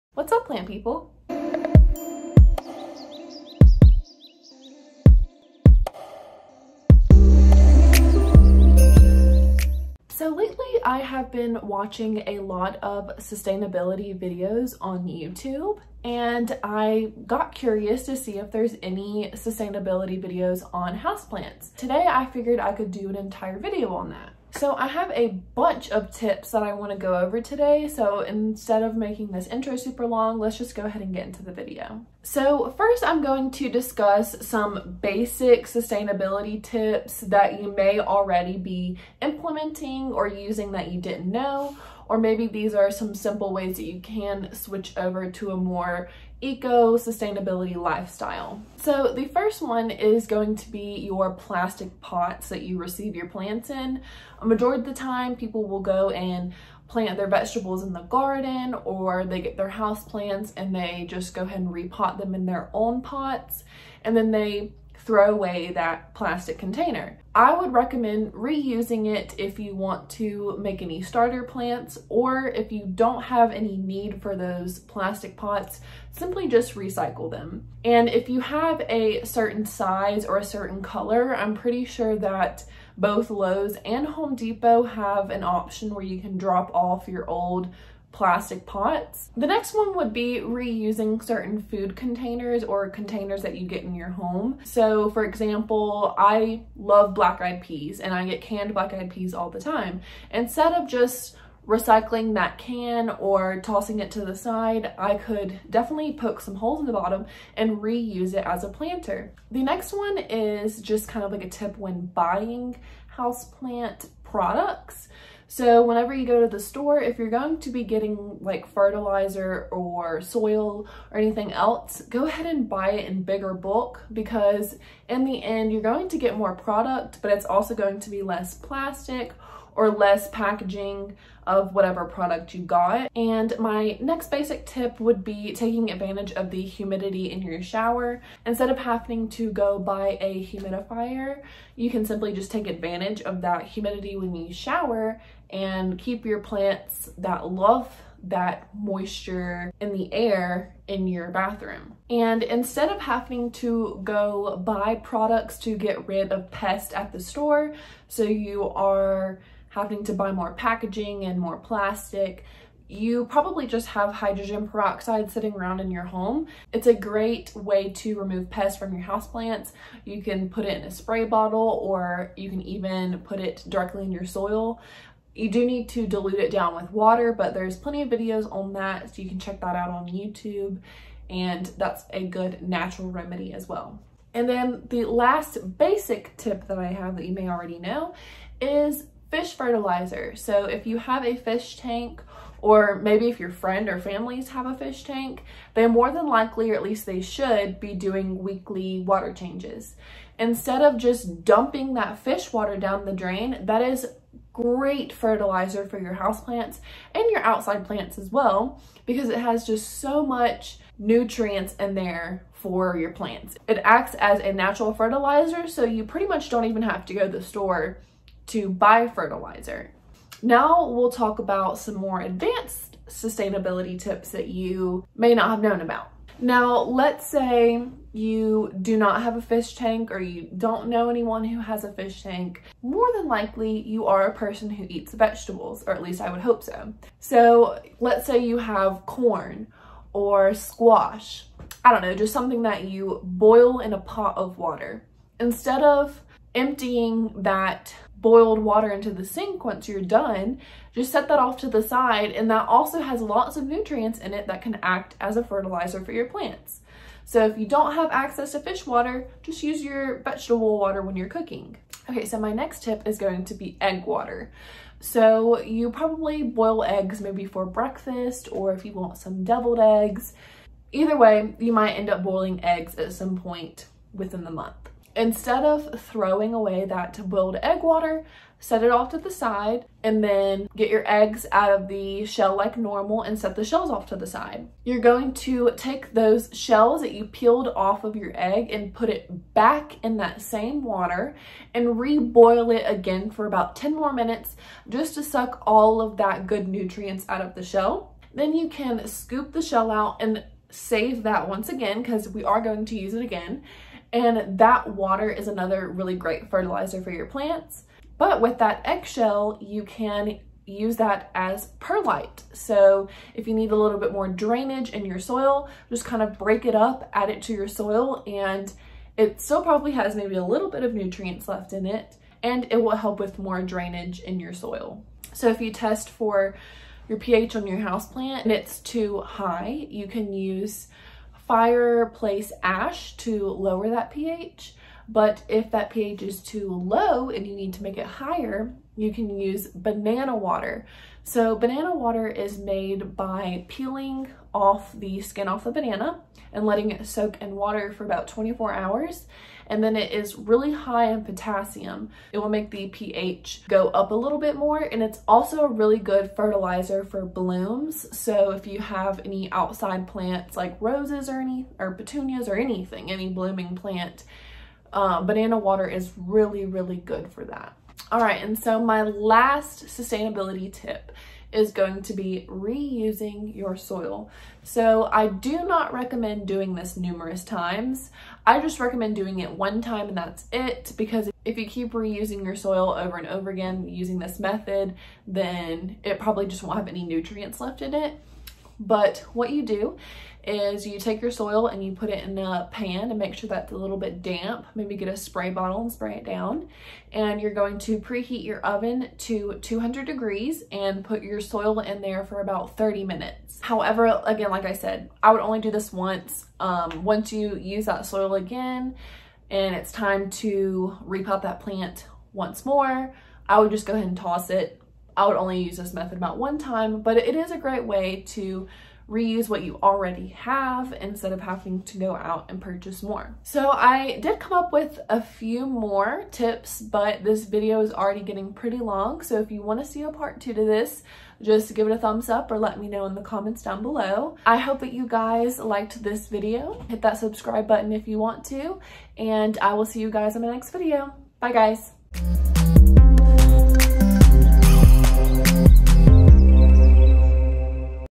Sustainability Tips for Houseplant Care!!
I hope you enjoyed this video! You can be sustainable in all parts of life, even houseplants! Try to incorporate some of these tips in your life to help our Earth out!